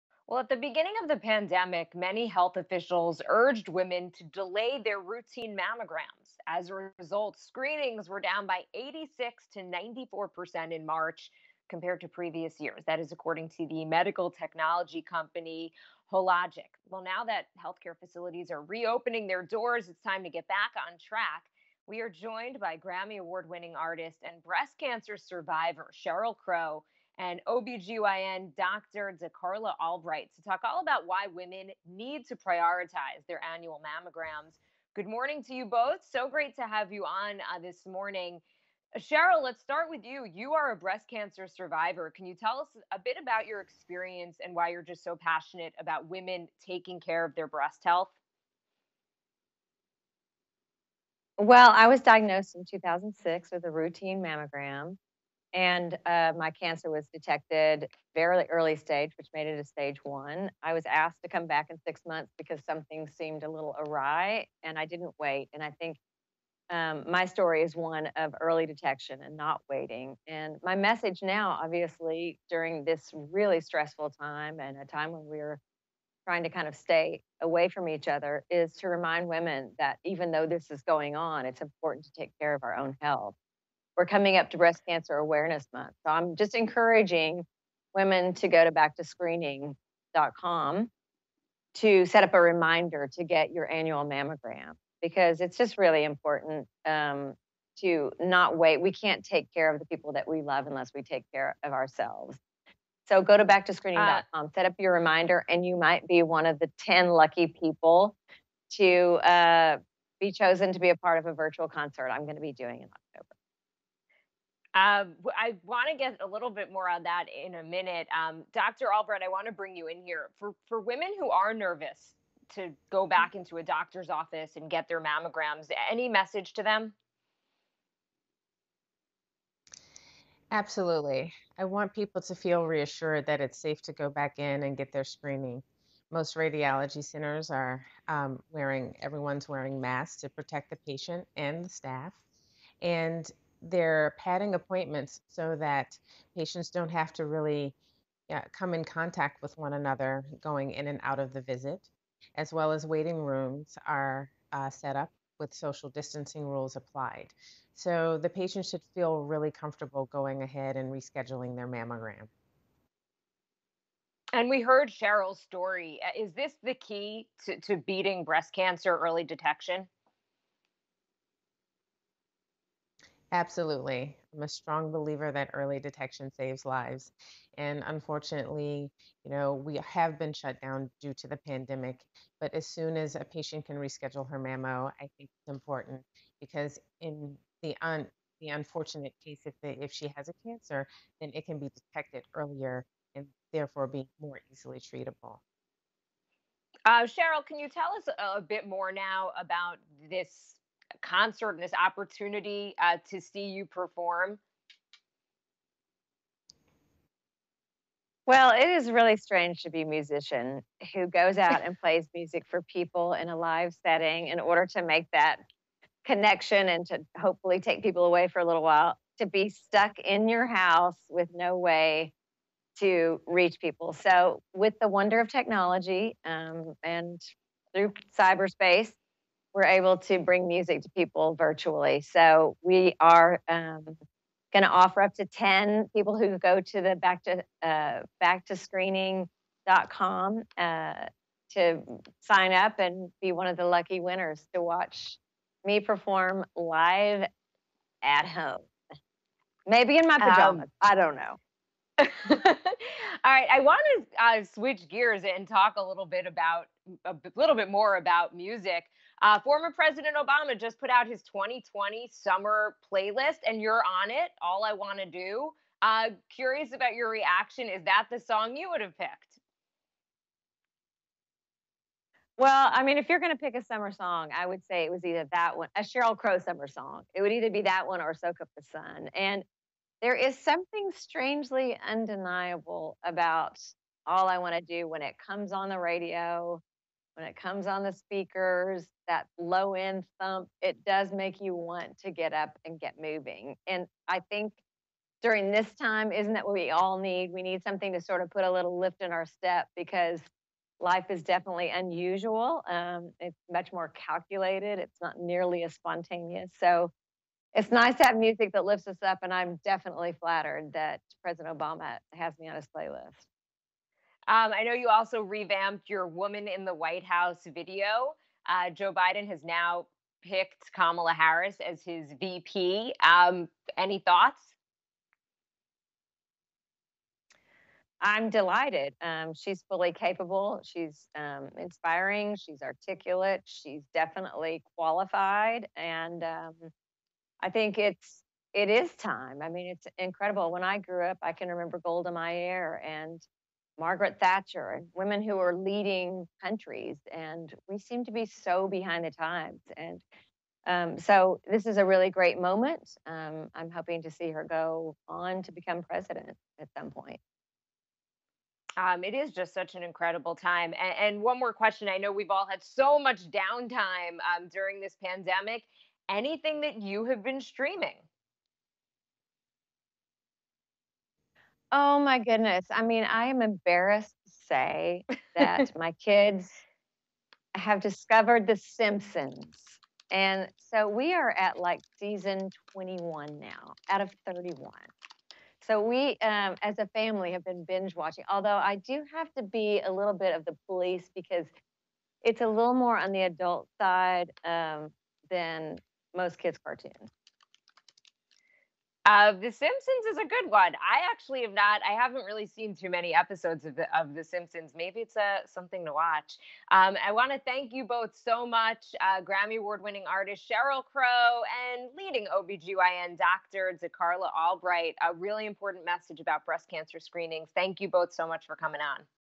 Grammy Winner Sheryl Crow Urges Women to Prioritize Breast Cancer Screenings
As the coronavirus pandemic began to spread in the U.S., many health officials urged people to avoid unnecessary medical visits, which meant delays for women's routine mammograms. Now that healthcare facilities are reopening their doors, the numbers are rising and advocates are encouraging women not to further delay their screenings.
"We can't take care of the people we love if we can't take care of ourselves," Grammy Award-winning artist and breast cancer survivor, Sheryl Crow told Cheddar. Also, her thoughts on landing a spot on former President Barack Obama's Summer 2020 playlist.